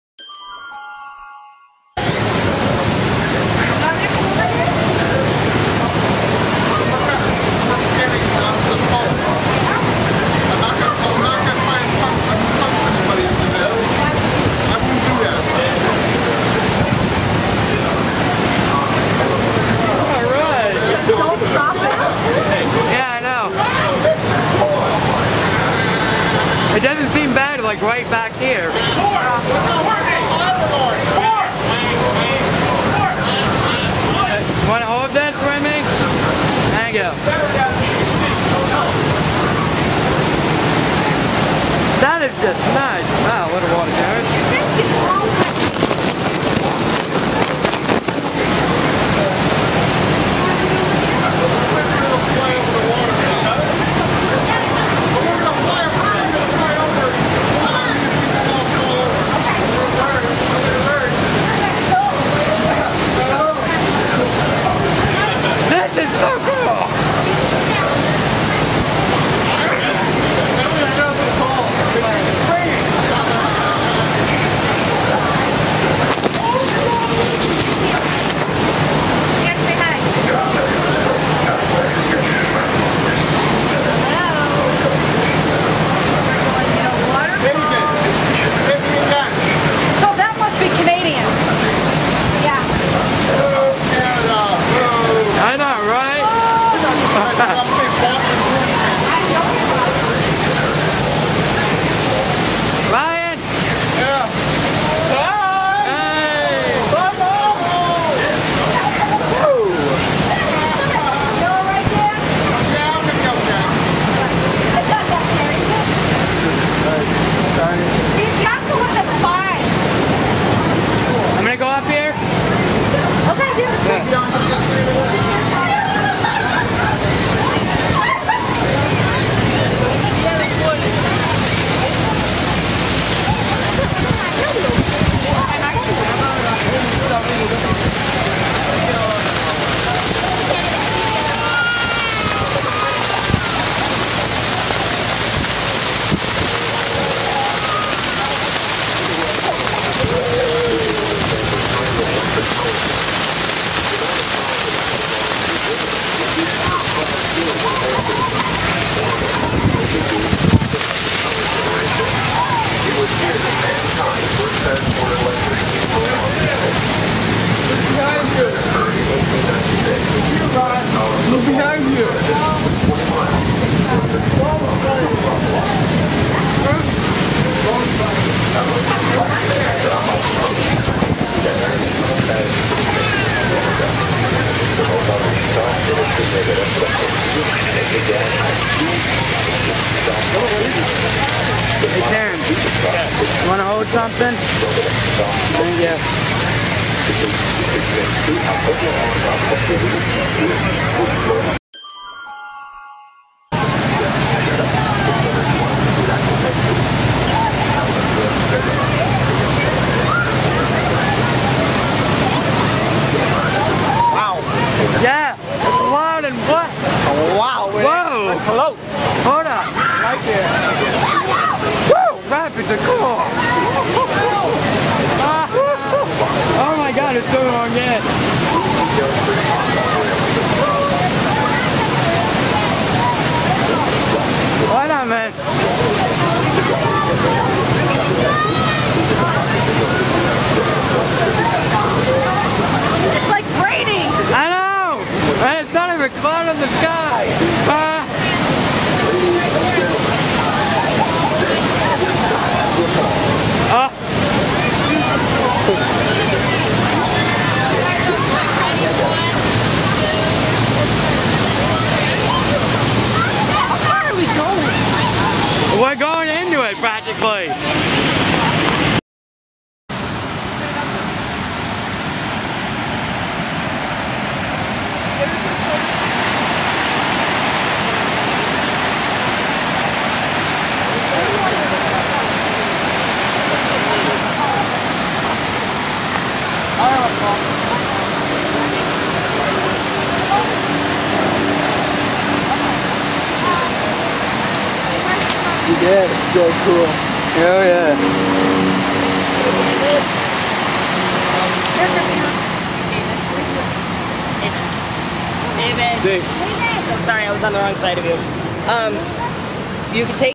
davidslife: niagarafalls newyork niagarafallsusa maidofthemist wny niagarariver
While also taking a first hand look at one of the wonders of planet Earth, Niagara Falls! The Falls is just that the wonder of the world. Its massive and makes you quite thirsty!

Niagara Falls is a group of three waterfalls at the southern end of Niagara Gorge, spanning the border between the province of Ontario in Canada and the state of New York in the United States. The largest of the three is Horseshoe Falls, which straddles the international border of the two countries.

The smaller American Falls and Bridal Veil Falls lie within the United States. Bridal Veil Falls is separated from Horseshoe Falls by Goat Island and from American Falls by Luna Island, with both islands situated in New York.

Horseshoe Falls is about 57 m (187 ft) high,[5] while the height of the American Falls varies between 21 and 30 m (69 and 98 ft) because of the presence of giant boulders at its base.

The current rate of erosion is approximately 30 centimeters (1 ft) per year, down from a historical average of 0.91 m (3 ft) per year. At this rate, in about 50,000 years Niagara Falls will have eroded the remaining 32 km (20 mi) to Lake Erie, and the falls will cease to exist.

?sub_comfirmation=1